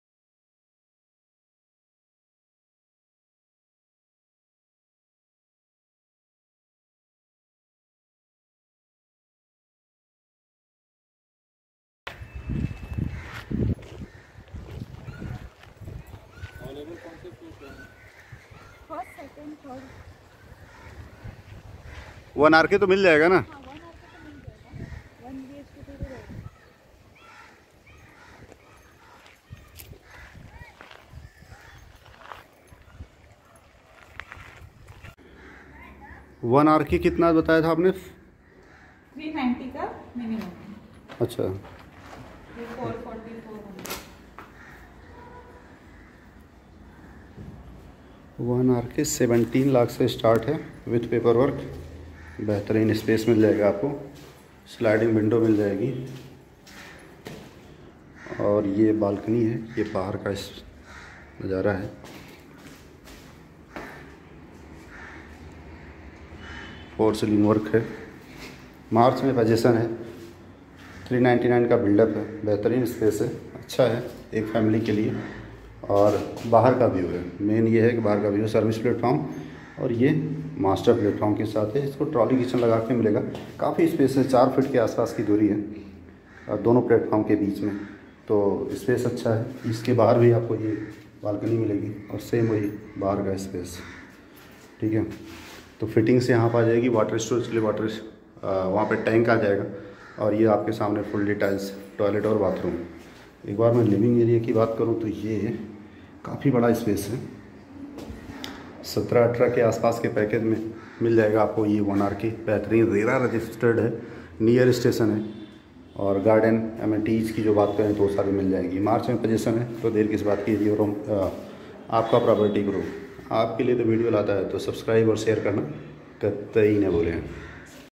1Rk Flat For Sale In Naigaon|| Only 17Lc ₹
1Rk flat in Naigaon Near Station

390 sqft buildup
17Lc₹ paper work
430 sqft Build up
18Lc₹ paperwork

Near, Bank, Juchandr station 2min, Market, Riksha stand, Naigaon 10mn, .etc

Rera Project,
Any Bank Loan available
90% Loan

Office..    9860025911